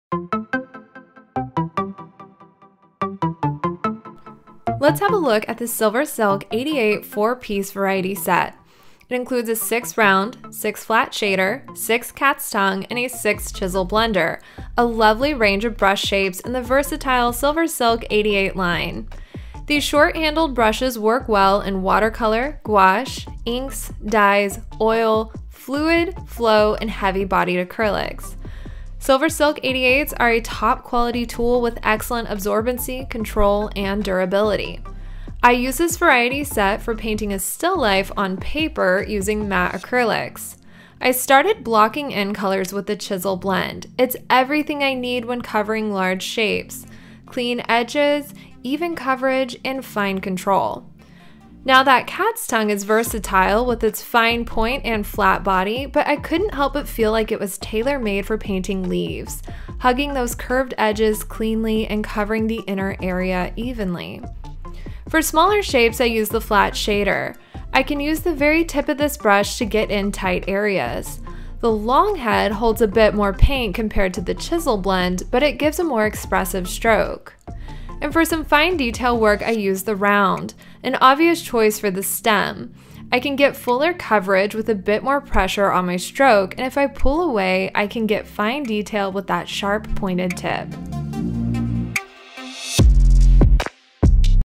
SK-8106 Silver Silk 88® 4pc Set
Silver Silk 88® is one of the only brushes on the market that was specifically designed to capture and carry low viscosity paints like liquid, fluid or interference acrylic; soft bodied acrylic, watercolor, and gouache. High-quality multi-diameter Japanese synthetic filaments imitate the absorbency, spring and feel of expensive natural hairs at a fraction of the cost.

Affordable Luxury in an Artist’ Brush
Out works, out performs and out lasts all others
The most responsive brush for Fluid Acrylics,
 Interference acrylics, and all acrylics, watercolors, gouache & more

Silver Silk 88® has great absorbency, color control, wear resistant. It resists puddling and drips. Carries color reliably from palette to painting surface. Exceptional for wet on wet technique. A go-to workhorse of a brush. Make your mark with Silver Silk 88®.

The following comments are from artists we sample with the Silver Silk 88™ series. Each one has favorites, and all love the performance of this series in Fluid Acrylics.

Silver Silk 88® allows for a seamless application of color and a delicately textured stroke. Artists say these brushes are a joy to use because they glide so effortlessly across the painting surface.

Oil painters adore Silver Silk 88® because they can gently soften and blend colors
Expect a fine point, chiseled edges and unmatched durability.
Handmade by expert artisans.
Triple epoxy sealed nickel-plated brass ferrule ensures the longevity of your brush

Specific Way to Clean the Brush:

Be sure to clean your brushes after each painting session. Letting paint dry in the brush can cause it to not perform as expected.
Rinsing the head of the brush with warm water should be enough to remove most watercolor paints, gouache, inks or dyes but an artist-grade brush cleaner can be used to make sure all traces of pigment have been cleaned off.
NEVER leave your brush resting in water! This can cause the handle to crack.
Always lay your brush flat to dry.